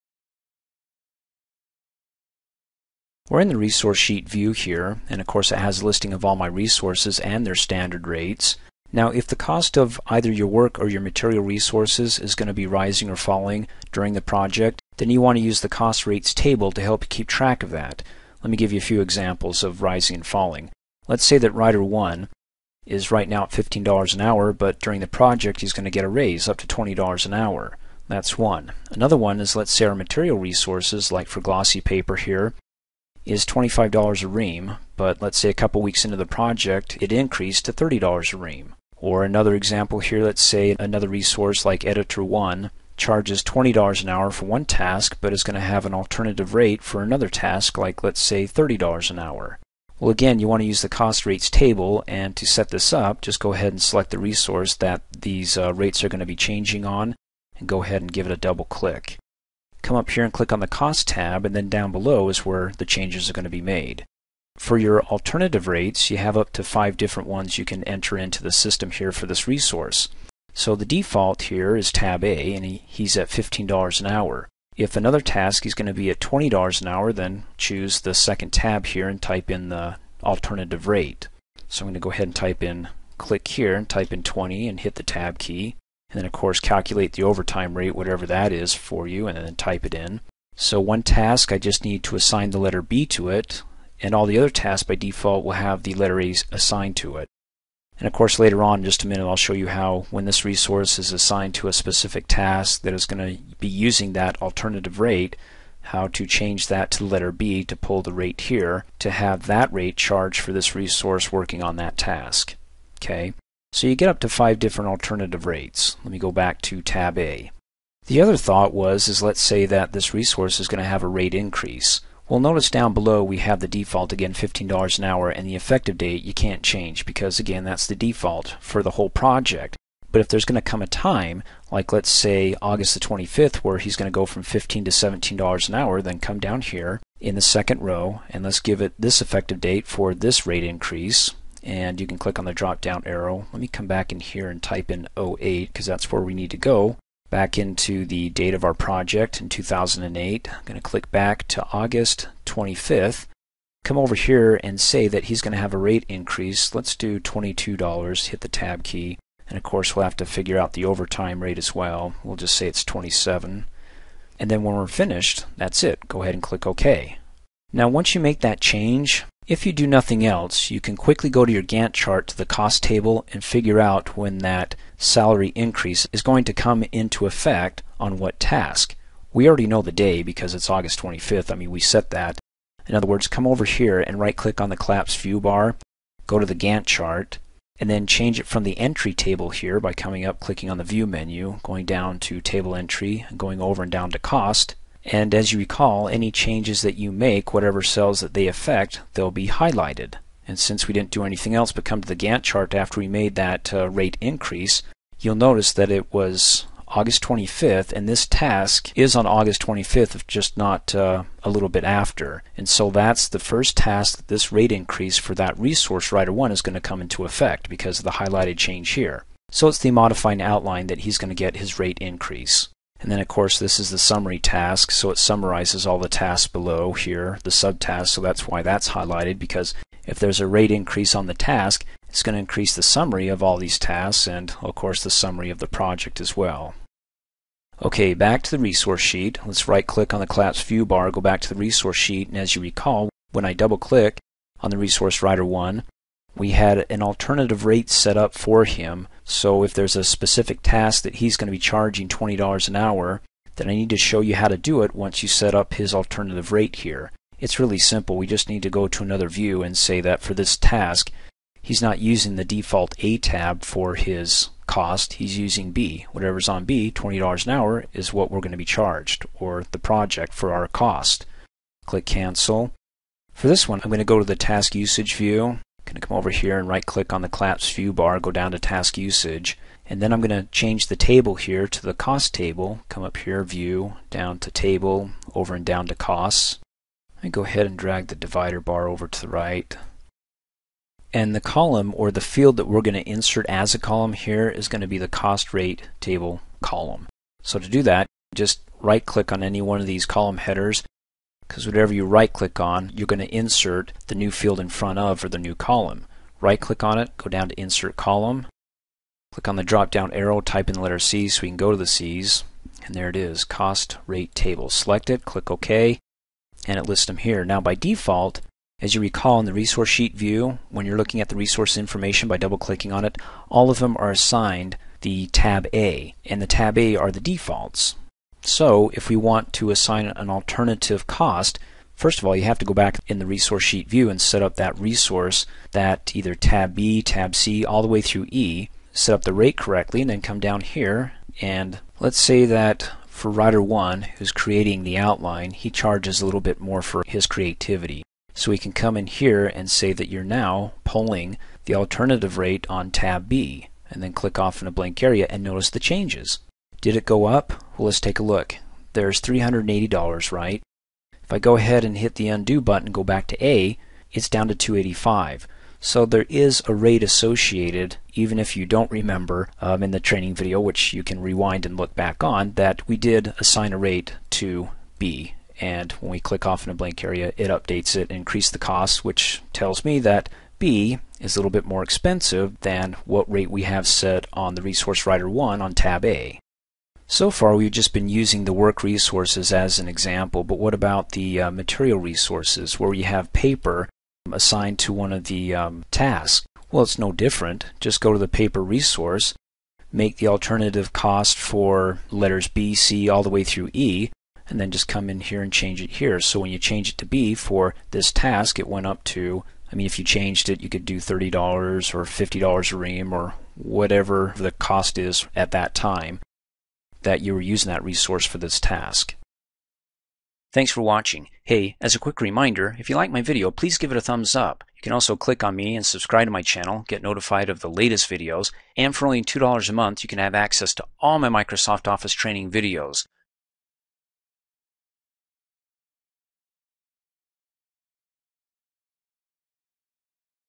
Project 2007: Cost Rates Table
Project 2007 tutorial on rising and falling costs, or fluctuating costs and how to keep track of them in the Cost Rates table.